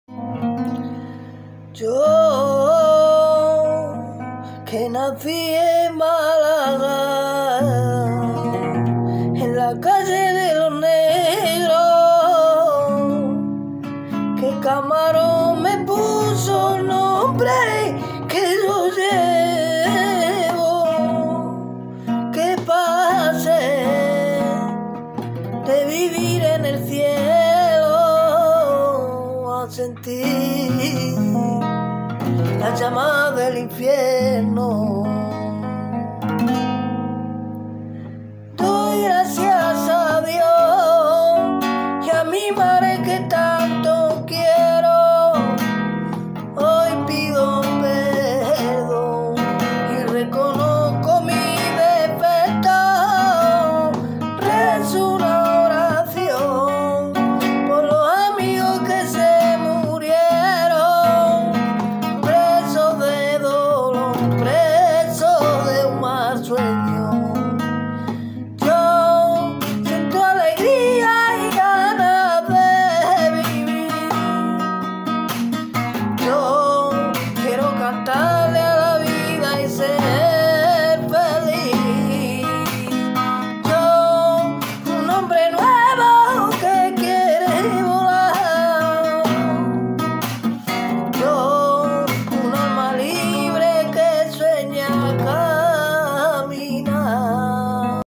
yo canción dedicada a tijeritas
Canción dedicada a tijeritas si os llega el vídeo y le veis al tijeritas ponérsela os lo agradecería mucho